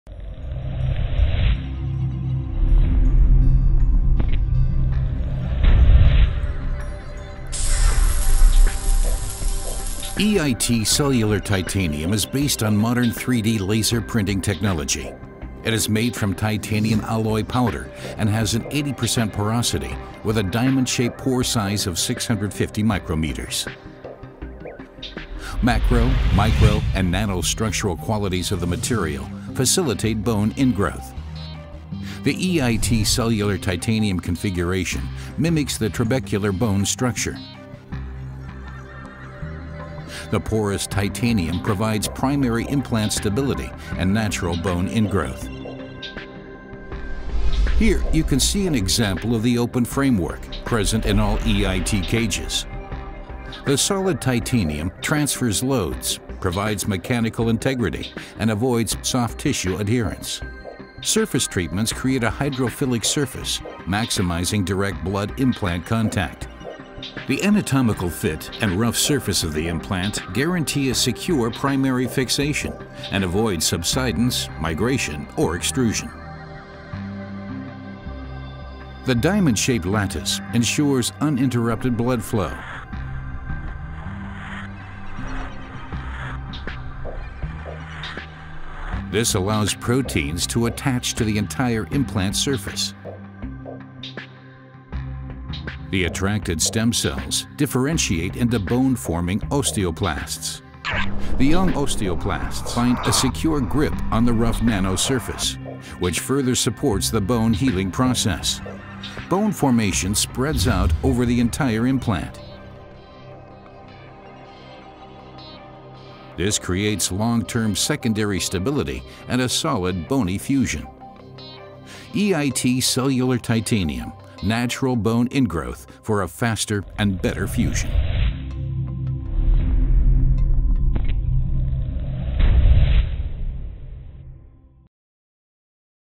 EIT CELLULAR TITANIUM® ANIMATION
Learn more about our EIT Cellular Titanium®. The state of the art porous titanium cages facilitate a fast and anatomical balanced fusion process.